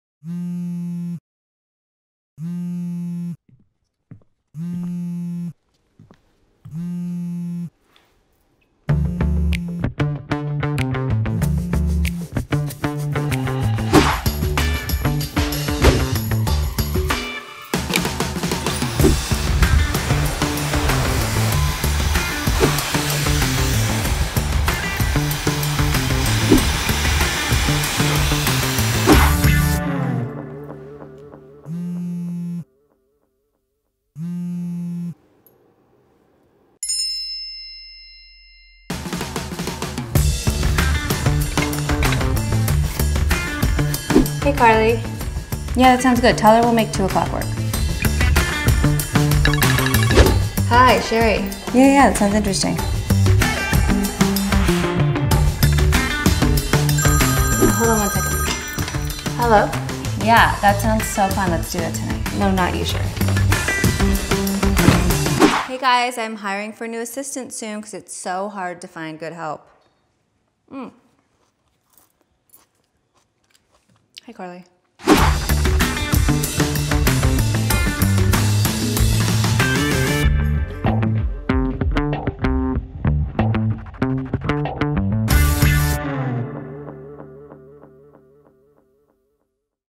Jen Atkin’s Morning Beauty Routine
She somehow manages to balance her duties as a stylist to a long list of clients you’re probably already well-acquainted with. In collaboration with Dyson.

Director: Matt Kazman
Photographer: Jake Rosenberg
Writer and Stylist: Meagan Wilson